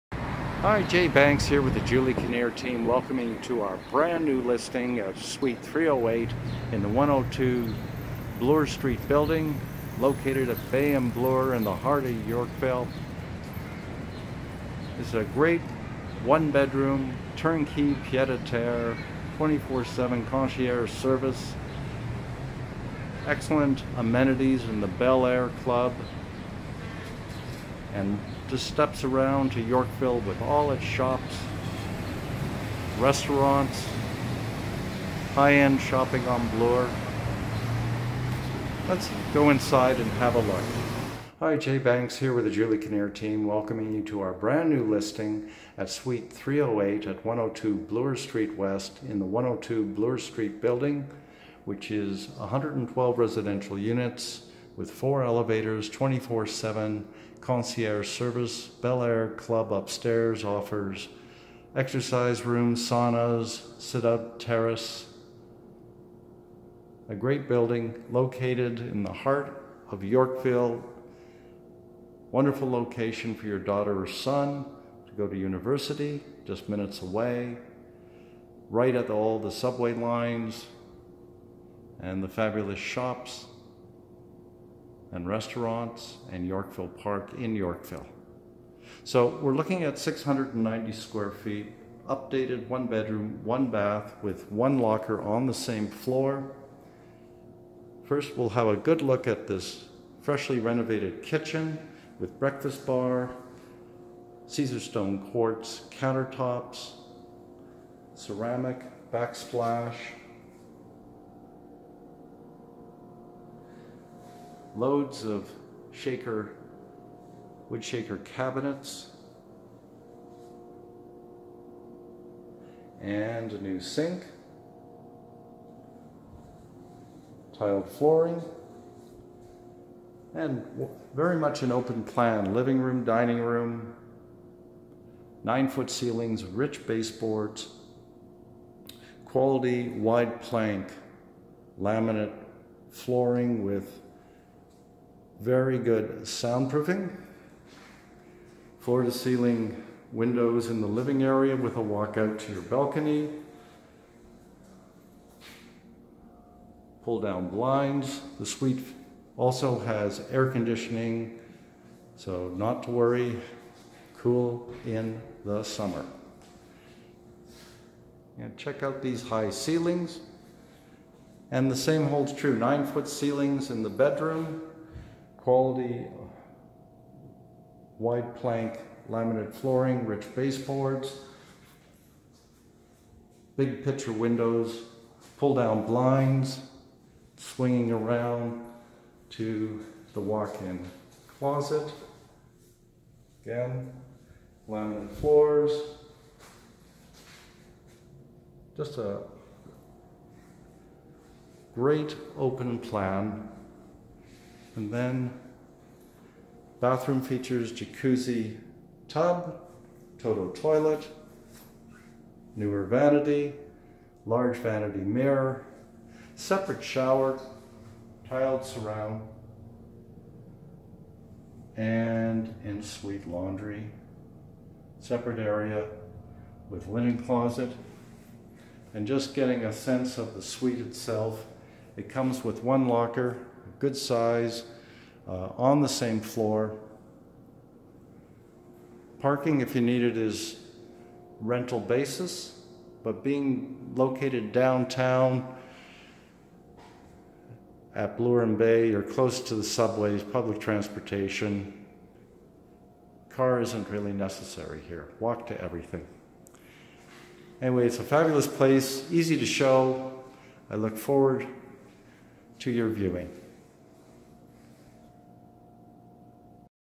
102 Bloor Street West #308 | Toronto - Yorkville | The Julie Kinnear Team
102 Bloor Street West #308 | Toronto - Yorkville, Toronto Real Estate For Sale

Pied à Terre in Yorkville - Turn Key Ready! Extensively Renovated in 2021

This spacious 1 bedroom starter home with 690 square feet of living space holds endless possibilities for the new owner. It can become a safe place for your child to live while they are attending university. Or it can be used as a base of operations to explore the city and start your career. Or keep it as a Pied à Terre when empty-nesting calls you abroad, and you want to have a place to come home to after your travels!